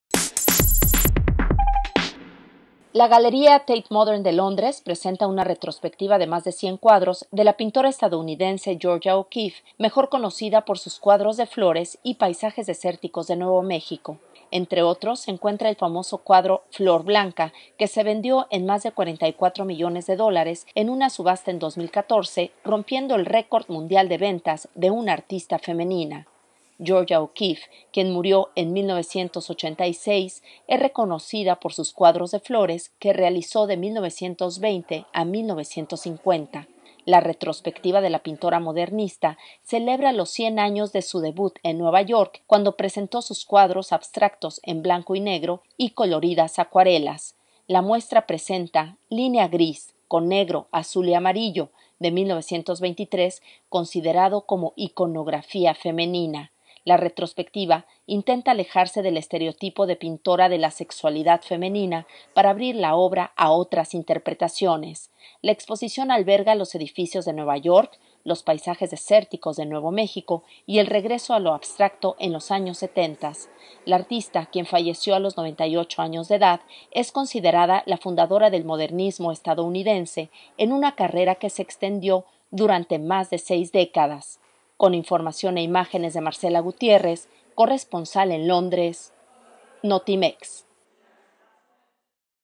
A 100 años de su debut, retrospectiva de la pintora Gerogia O'Keeffe en Londres
La galería Tate Modern de Londres presenta una retrospectiva de la pintora modernista Georgia O'Keeffe